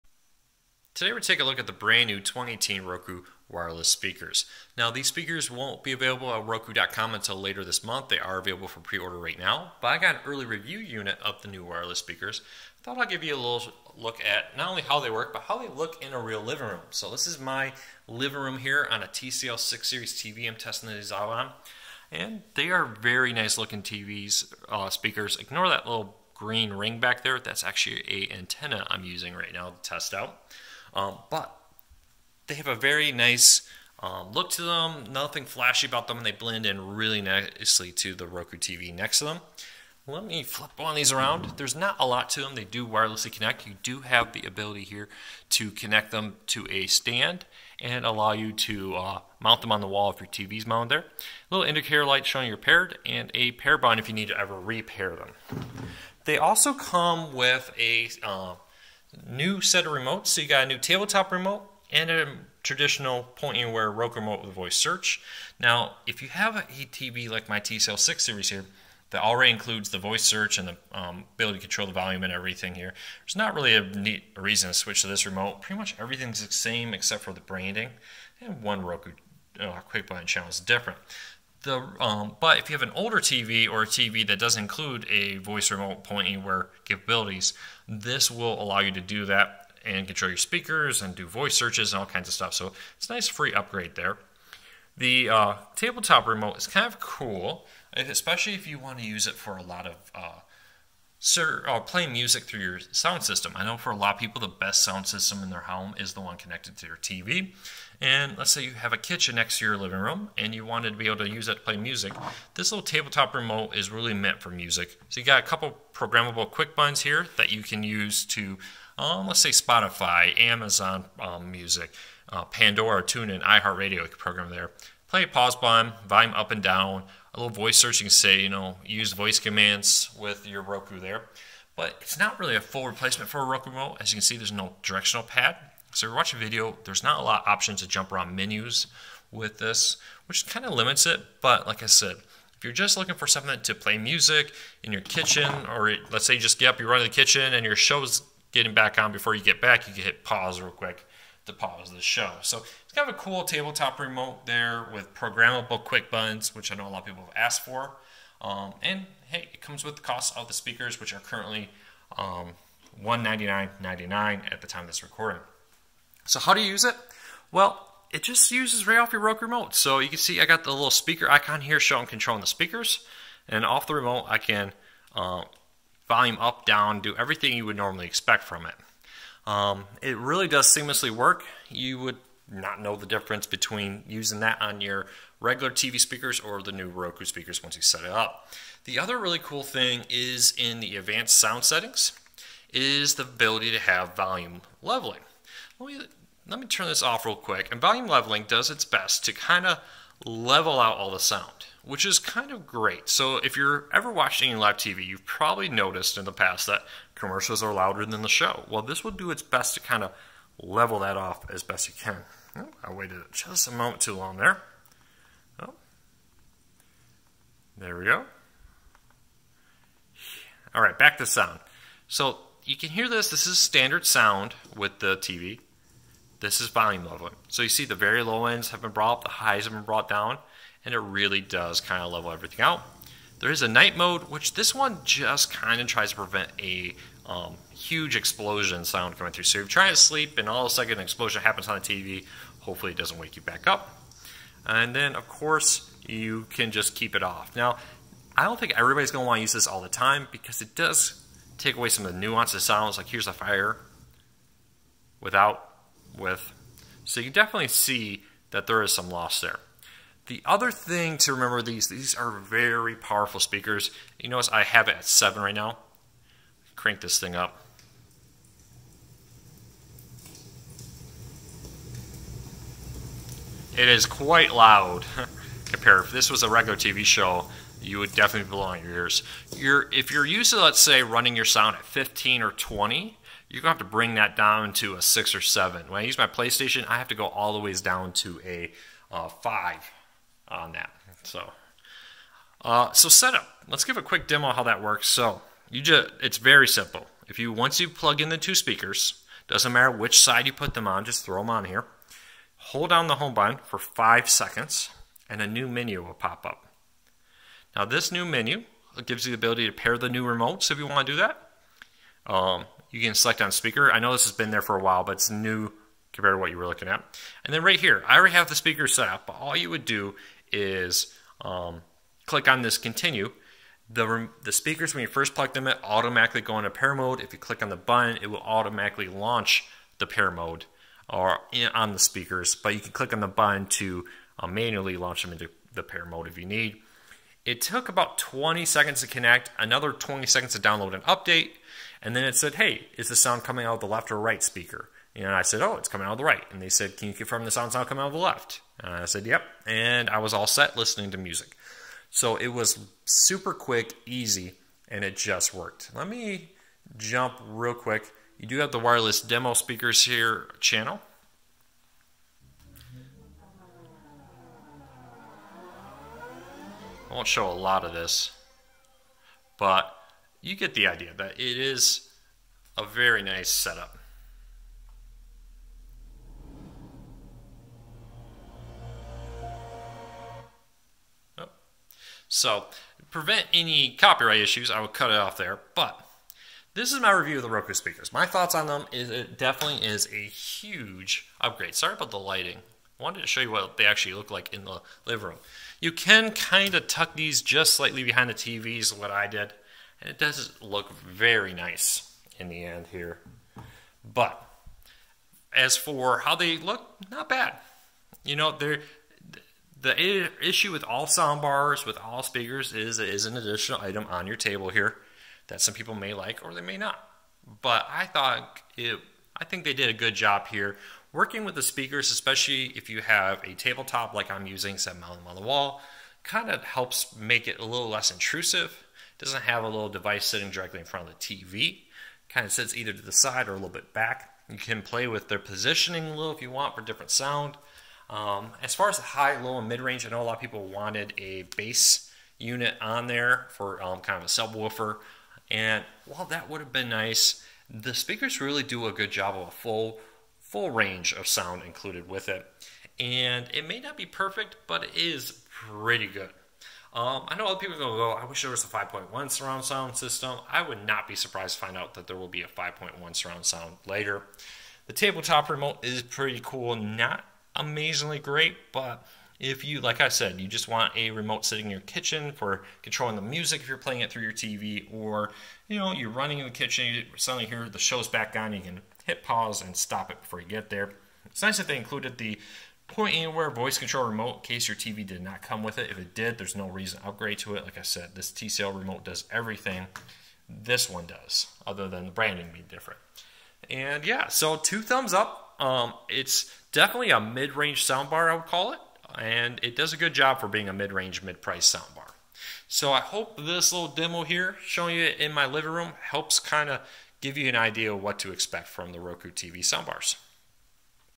Review: The 2018 Roku TV Wireless Speakers
Today we take a look at the 2018 Roku TV Wireless Speakers. These new speakers wirelessly connect to your Roku TV to offer a greatly improved sound experience comparable to most mid range soundbars. You can find our full written review on our

Quick note the video quilty. To try and give the best real world review I decided to record this in my home to show you how the speakers look set up in a real living room vs a well light review lab. Hopefully, the more real world test can make up for some of the video quilty issues.

We now have t-shirts and other products for sale!